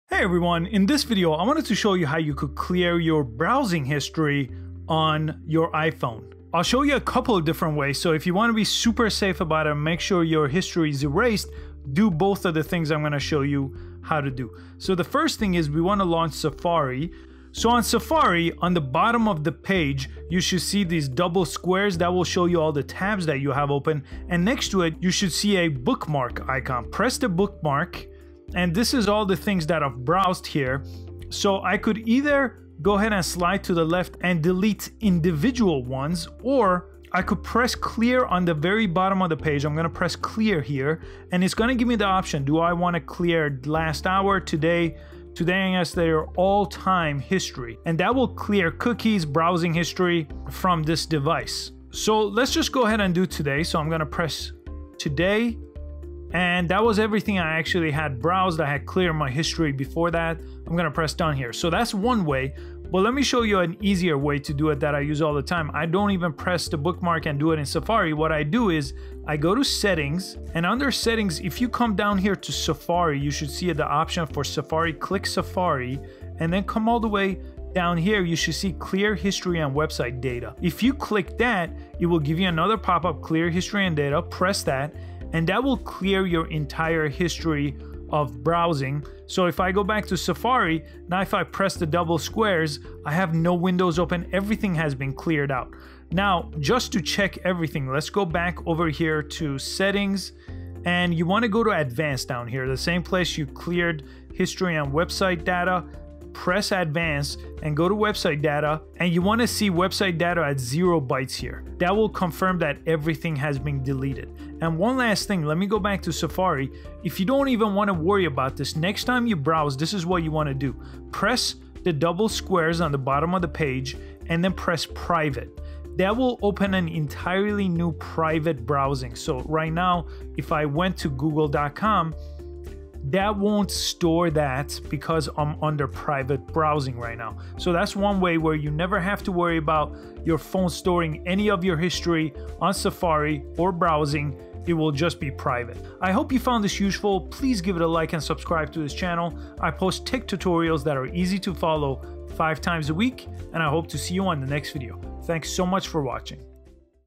How to Clear Browsing History on iPhone or iPad
Need to clear browsing history on iPhone or iPad? There are two options in iOS to clear your browsing history.

First is to the clear option in the bookmark tab of Safari,

The second option is using the general setting and the safari option which gives you clear browsing history and cookies.

In the future, you can use the private browsing option in Safari to make sure your search doesn't get saved on your phone.